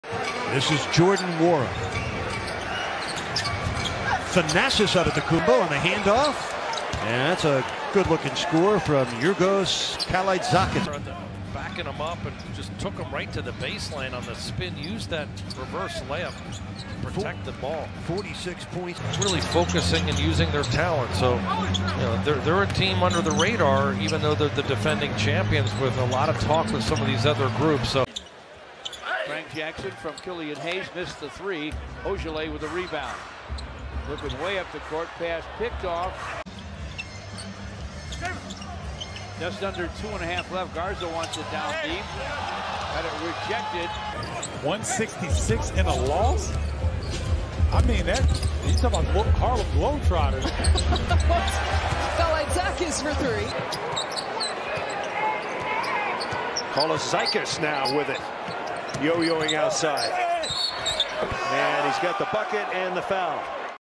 Georgios Kalaitzakis Full Rookie Highlights (21-22 Bucks)
Georgios Kalaitzakis Highlights 21-22

16 points
1 steal
1 block